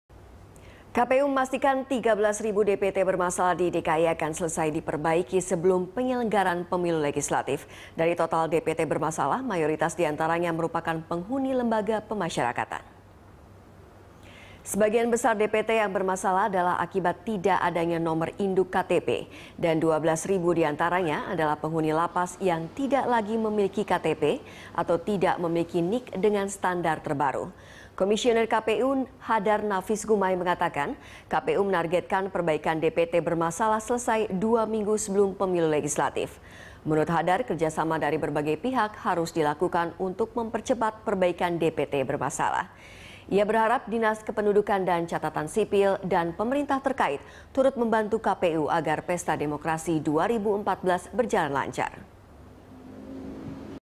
DPT Jakarta Rampung Sebelum Pileg
Komisi Pemilihan Umum (KPU) memastikan 13 ribu daftar pemilih tetap bermasalah di Jakarta akan selesai diperbaiki sebelum penyelenggaraan pemilu legislatif. Mayoritas DPT bermasalah adalah penghuni lembaga pemasyarakatan.

Facebook.com/BeritaSatuTV
Youtube.com/BeritaSatu